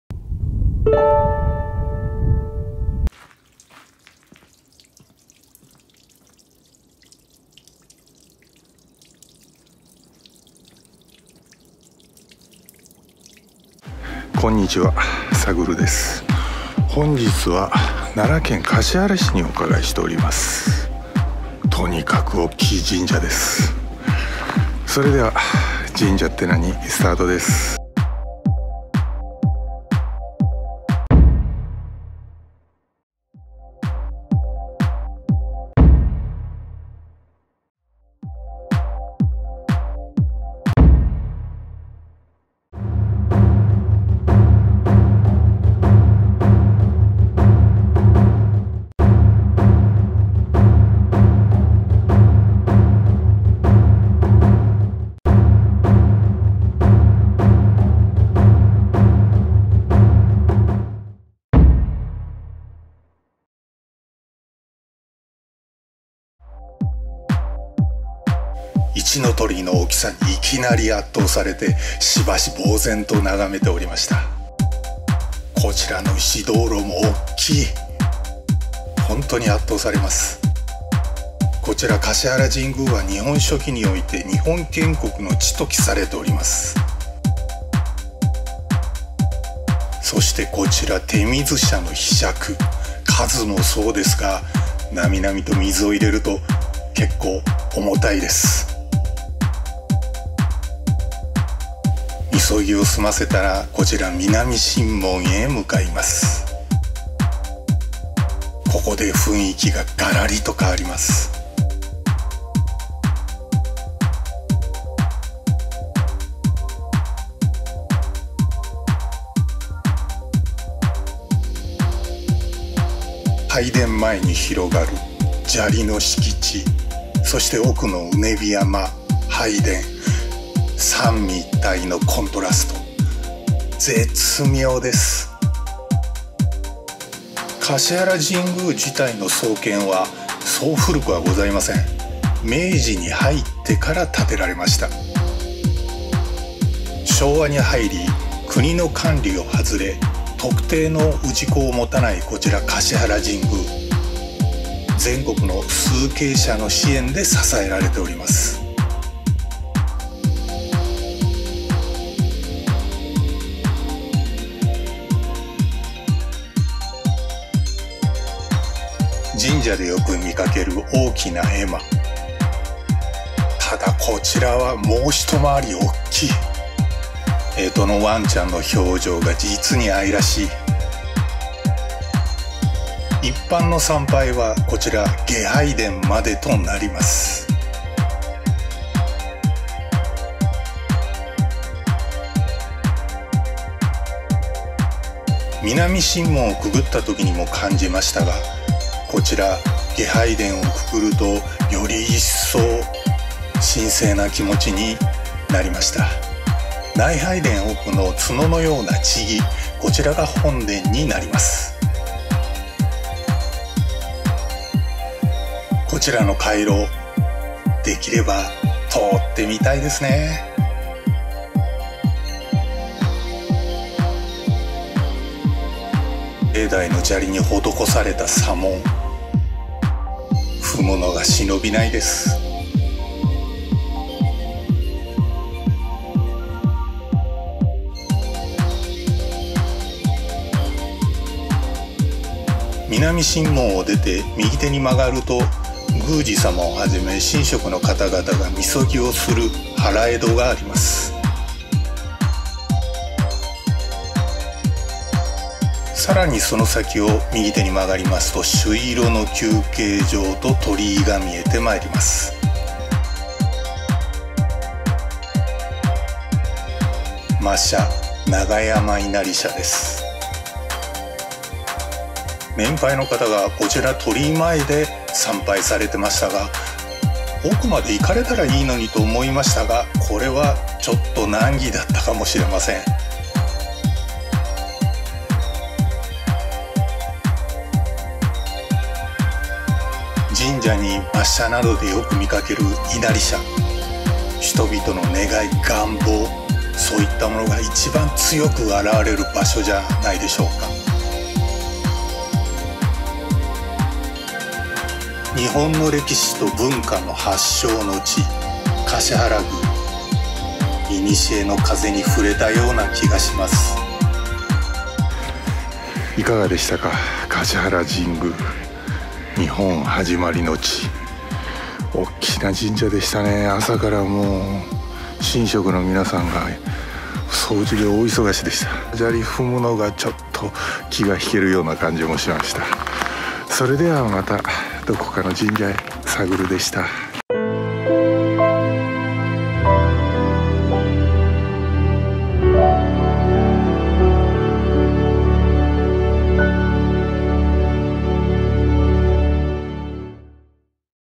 威風堂々 橿原神宮 【神社ってナニ？】 #45
橿原市の神社4回目は、日本の始まりの神社
橿原神宮大きさの中に繊細さを感じる境内必見です！